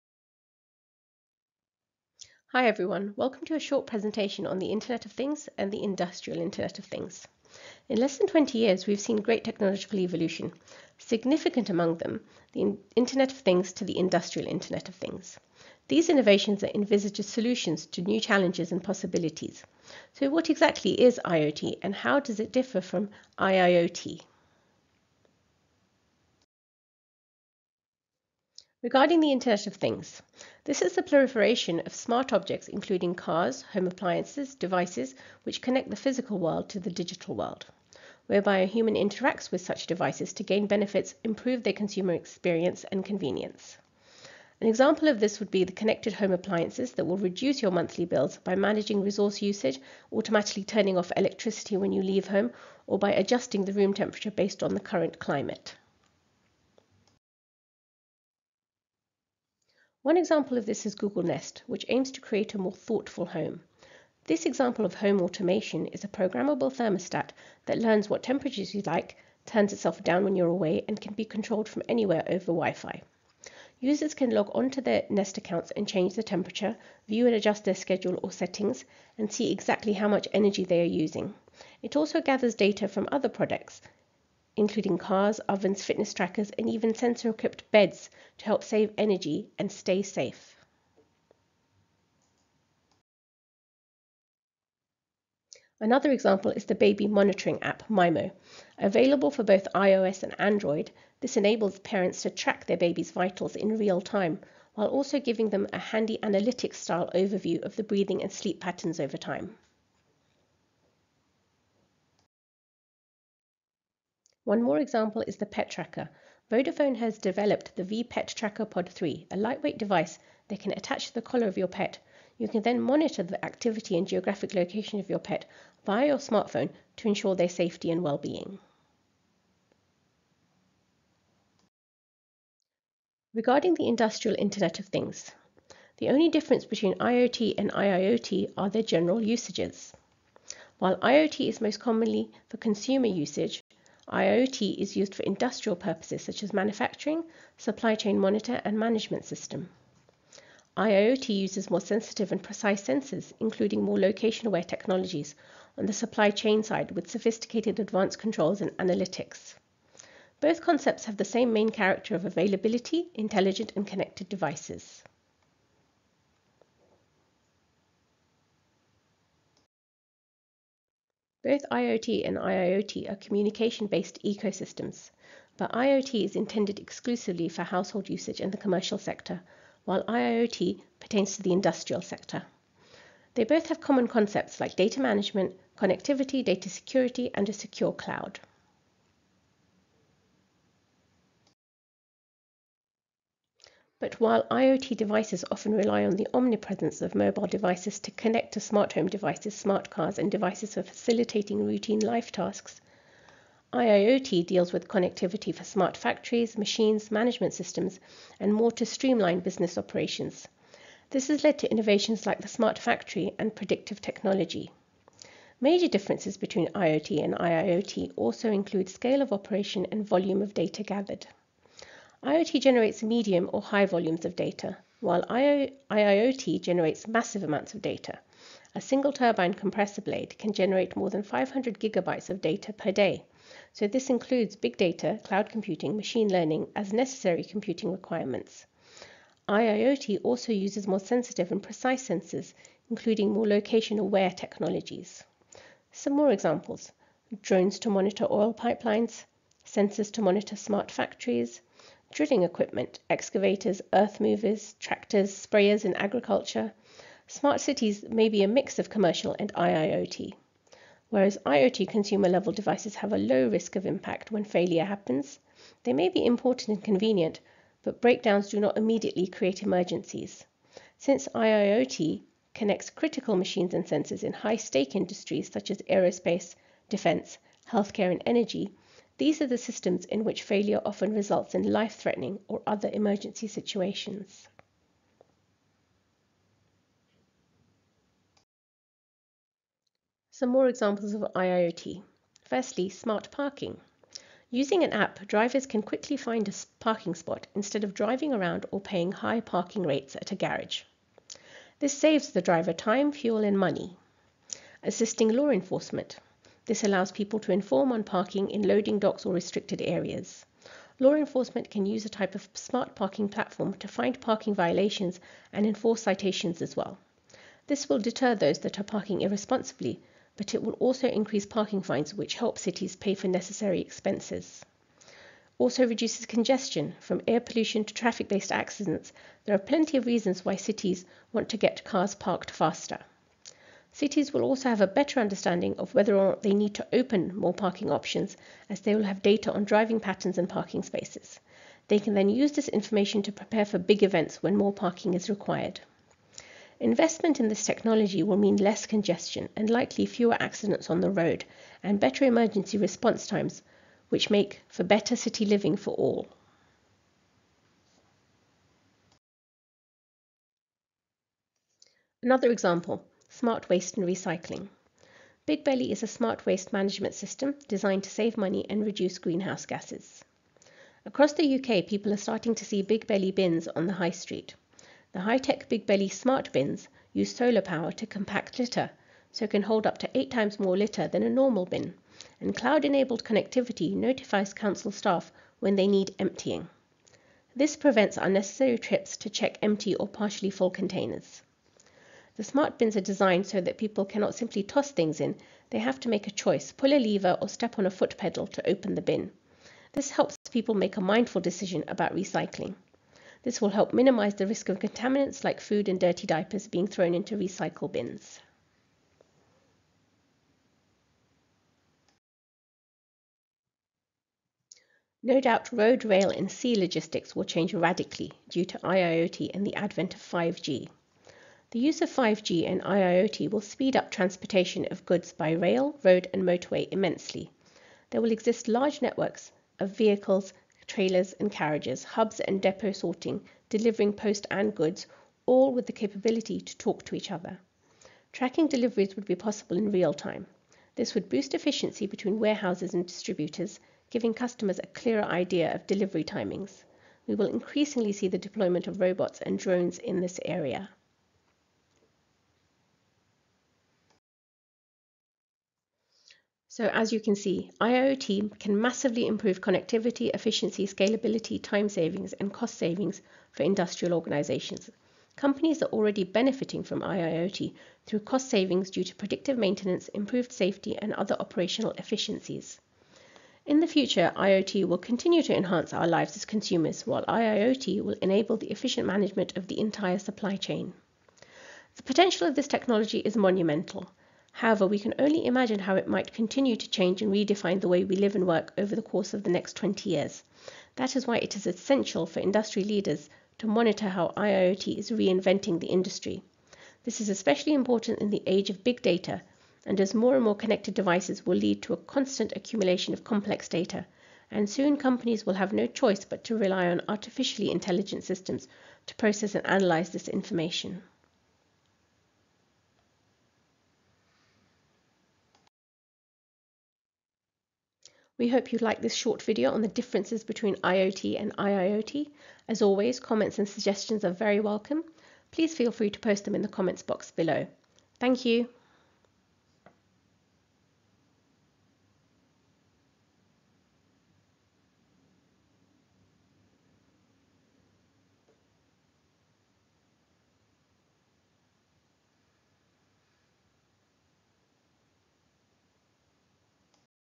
Beginners: What is Industrial IoT (IIoT)
An introductory video and presentation looking at Internet of Things (IoT) and differences between IoT and IIoT. Examples are provided to help clarify the understanding.